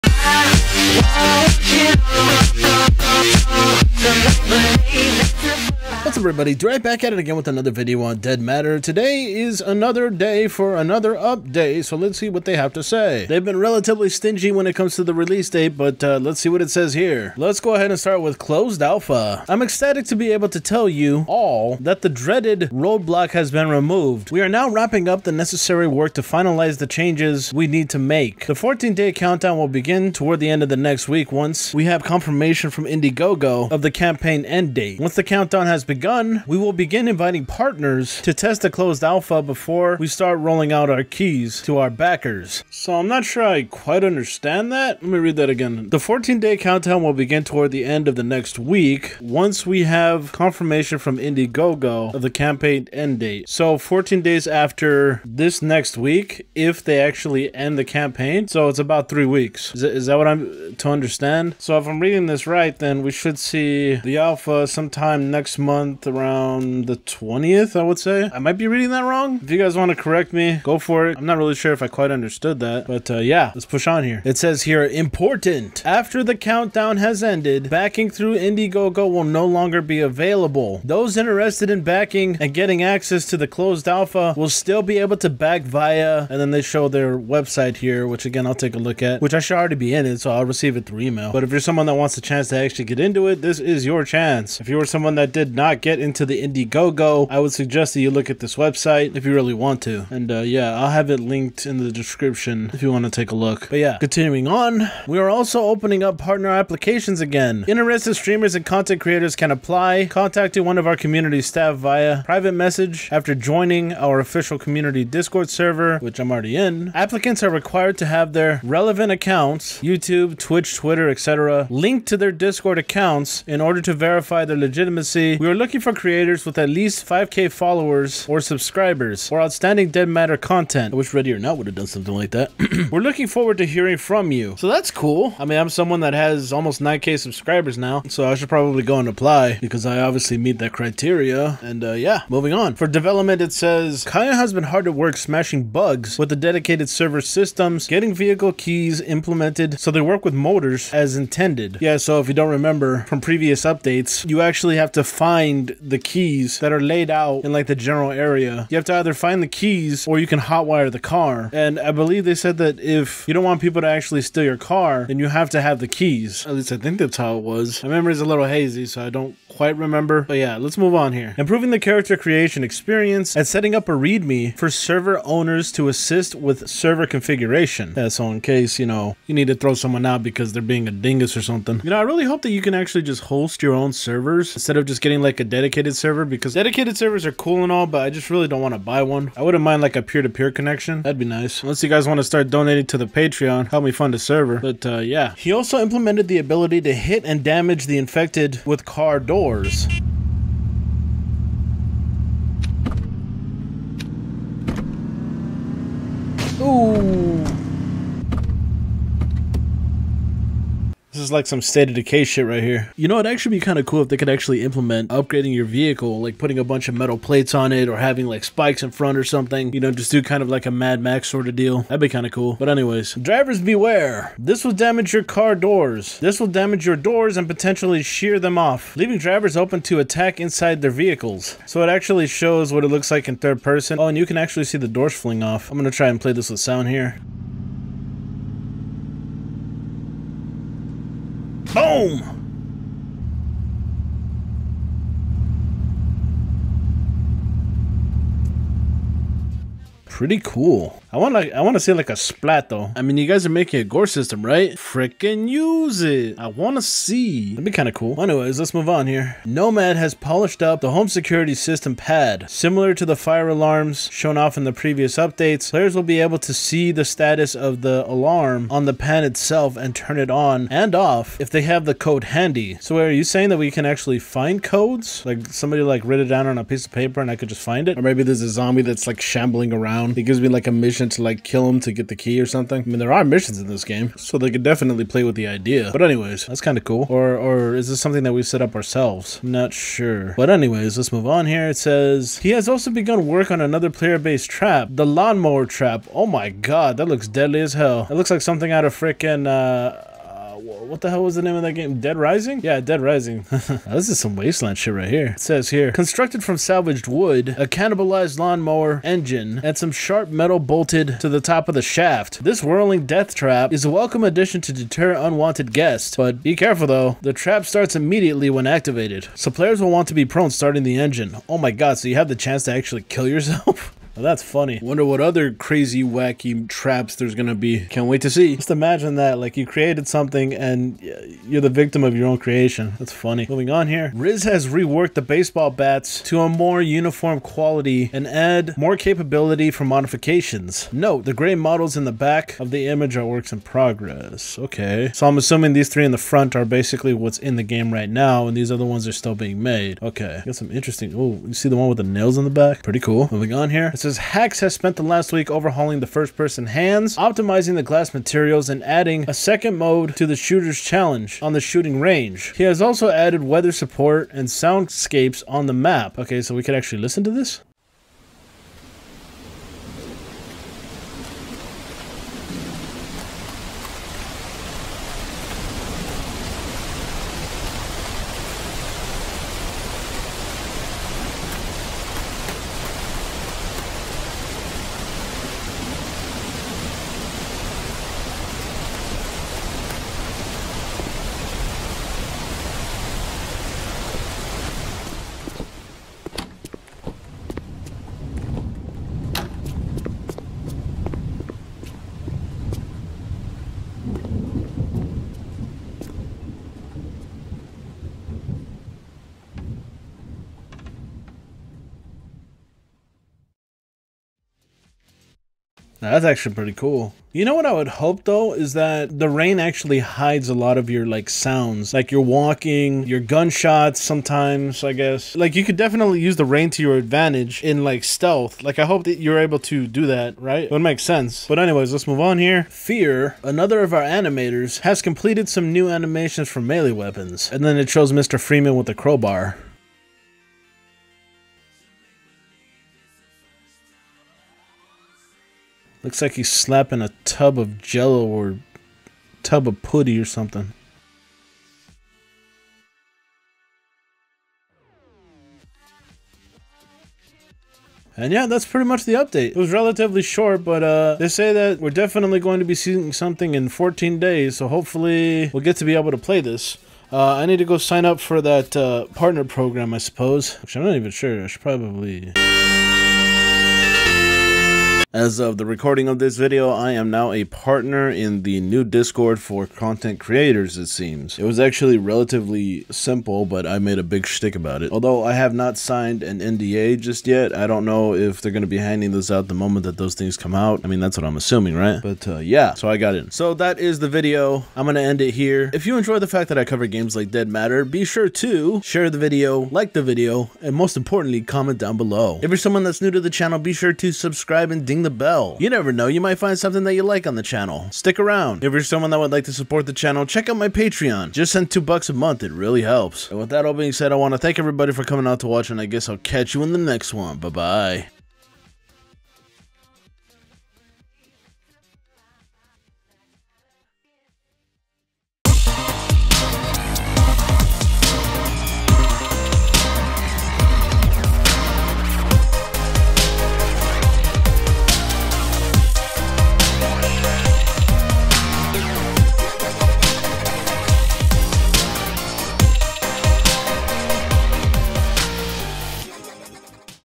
Dead Matter News & Updates - Ramming Zombies - CLOSED ALPHA JUST WEEKS AWAY!! & I Became Partnered!!
What Inspired This video?

They had multiple updates so I thought I would combine them into one video!

What is Dead Matter?

Dead Matter is a true sandbox survival horror. Players will fight to survive in a zombie-packed, post-apocalyptic world that fosters whichever play-style fits you best. Settle down and defend your home from outside threats with an expansive crafting and barricading system, cultivate and live off the land, or branch out, explore, and scavenge whatever vehicles, weapons, and food you may find in a zombie-plagued Alberta - alone, or with your friends. Dead Matter is a community driven, crowd funded survival game that aims to quench the thirst for a true zombie survival sandbox. Initially funded in August of 2017. We aim to bring the game to Steam Early Access shortly after our closed testing period.

Dead Matter caters to a multitude of survival playstyles that can be mixed and matched, affording players an infinite amount of ways to survive in post apocalyptic Alberta.

We've been hard at work over the past 9 months to bring Dead Matter to life. After many hardships the foundation has been laid and we're now working on fleshing out key content. We always listen to feedback that our fans give us and truly appreciate all of the support we've been shown thus far.

Songs Used;